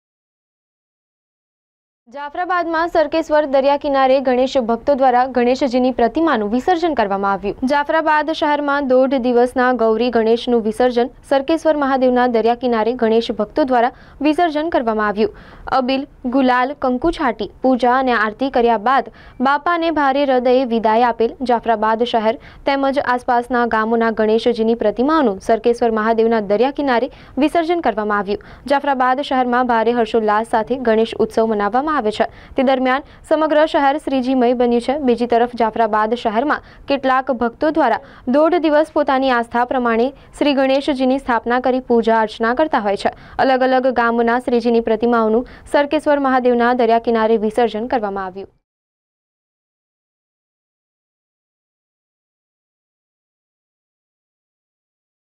જાફરાબાદમા સરકેશ્વર મહાદેવ ના દરીયા કીનારે ગણેશ ભકતો દ્રારા વિસર્જન કરવામાં આવ્યું 2024
જાફરાબાદમા  સરકેશ્વર મહાદેવ ના દરીયા કીનારે ગણેશ ભકતો દ્રારા વિસર્જન કરવામાં આવ્યું